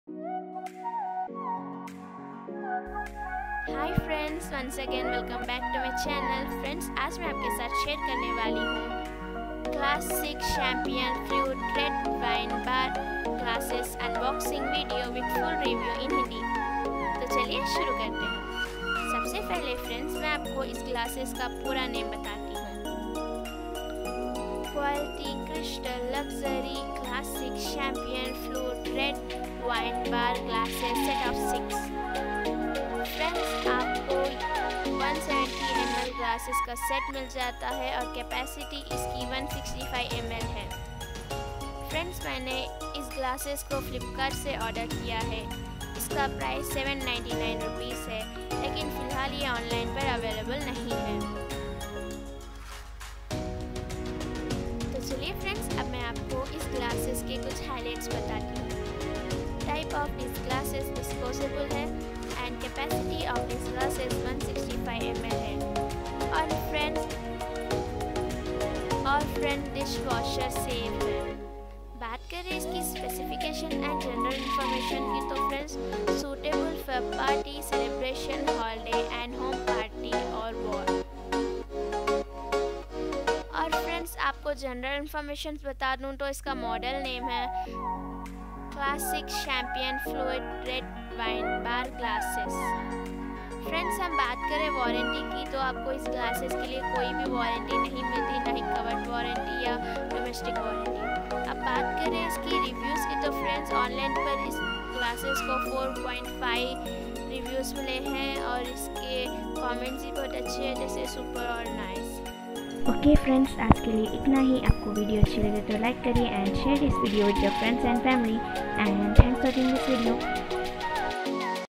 beautiful red wine champagne glasses with review in hindi
Best bar glasses review in hindi
beautiful red wine champagne glasses with review in hindi woth full review in hindi